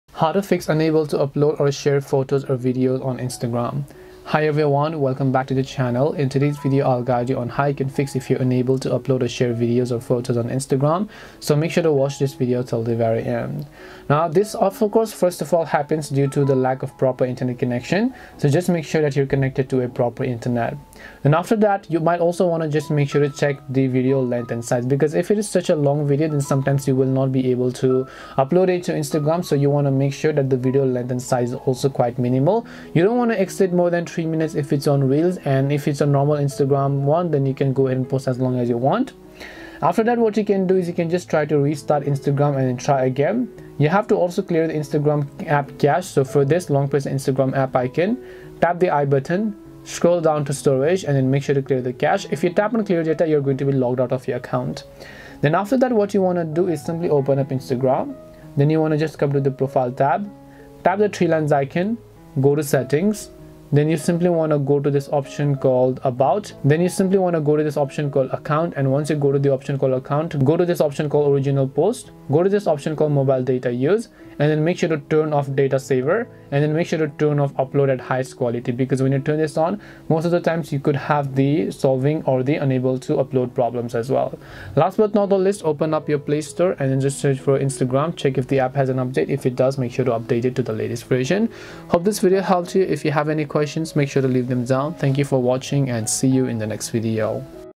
Unable To Upload or Share Photos /Videos - Instagram !
In this video, I will show you how to fix unable to upload or share photos and videos on Instagram.

~ Chapters:
0:00 Introduction
0:14 Why This Happens
0:20 Check the Video Length
0:41 Clear Cache / Data
1:05 Data Saver Off
1:25 Conclusion